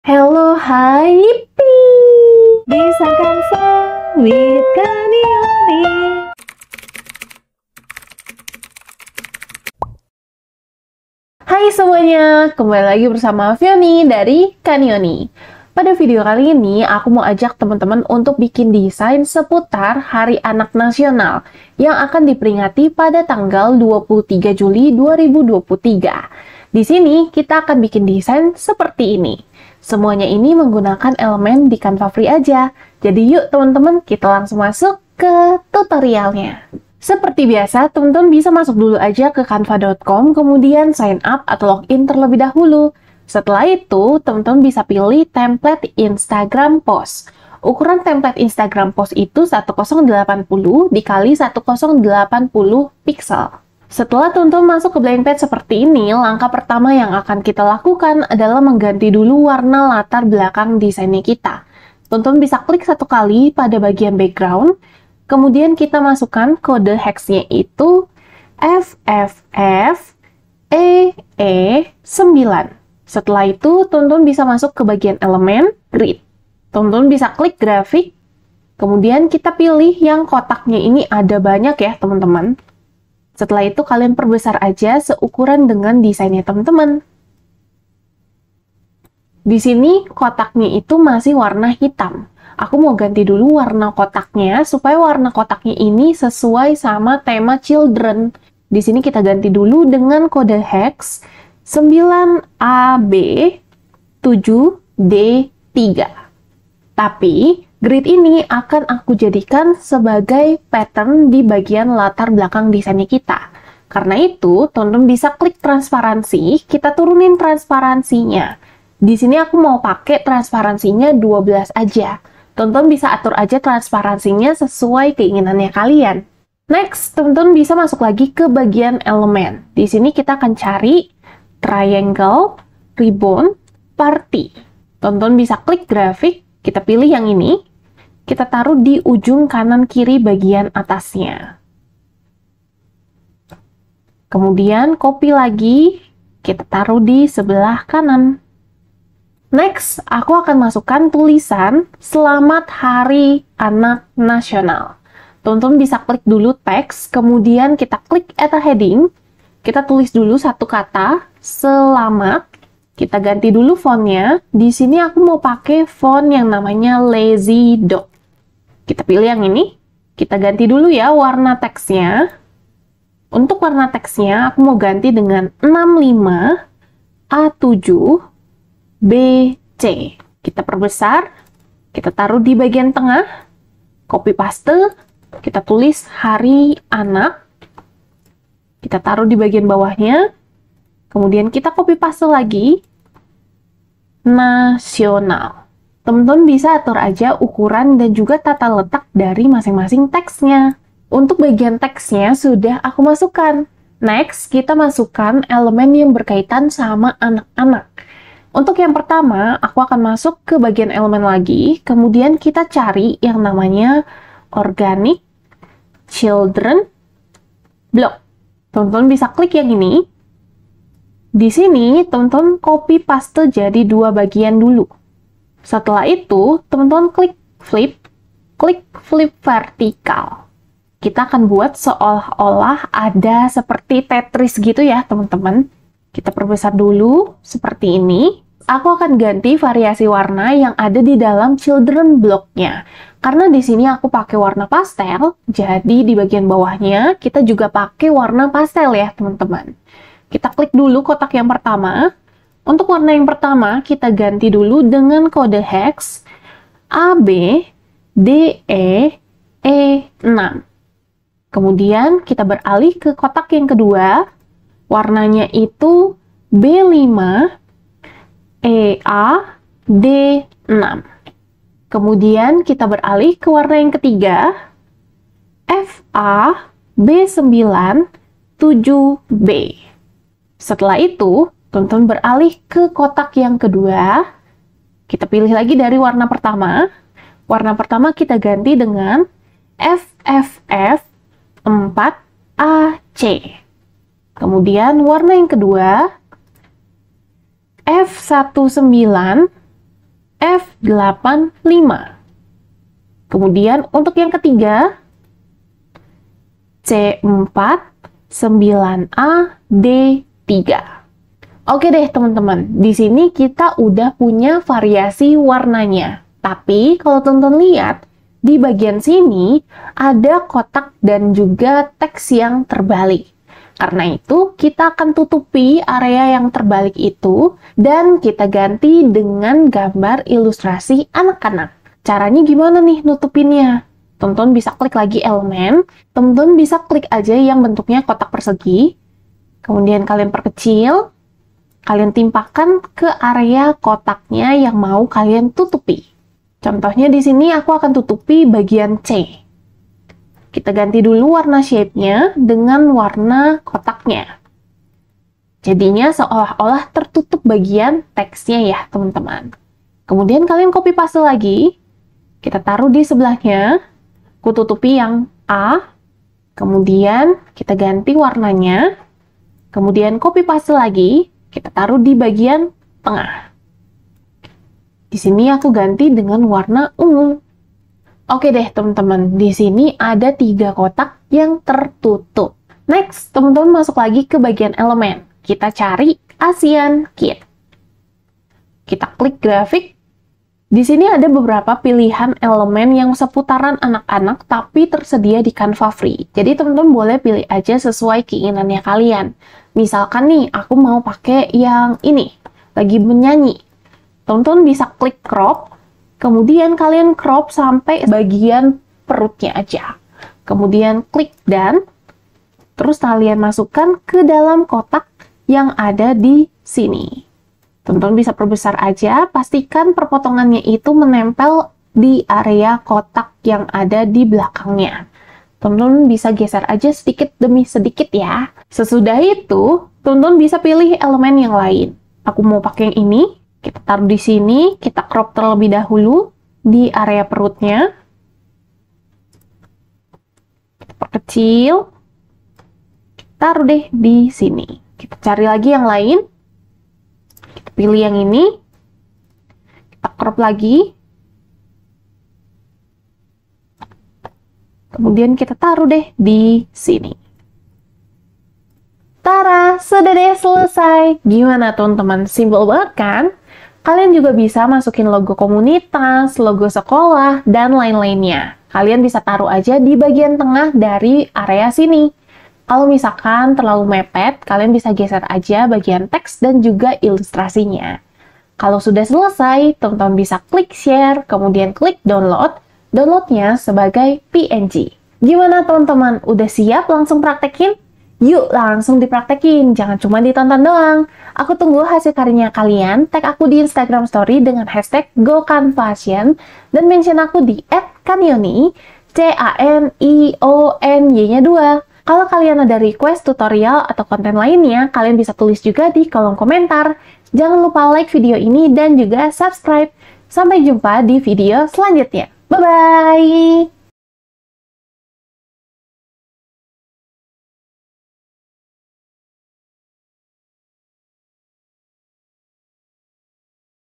Tutorial Desain Hari Anak Nasional (23 Juli)
Hai-halo dan annyeong!
Selamat datang di Youtube Canionyy yeaayy~

Desain kali ini merupakan desain untuk memperingati hari anak nasional di yang kita peringati pada tanggal 23 Juli. Desain kali ini simple banget dan ditambah lagi teman-teman hanya perlu menggunakan Canva FREE aja! Jangan lupa tag aku di IG atau tiktok @canionyy ya

Mau langganan Canva PRO? atau mau dapet ebook gratis?

Semoga kalian suka sama tutorialnyaaa~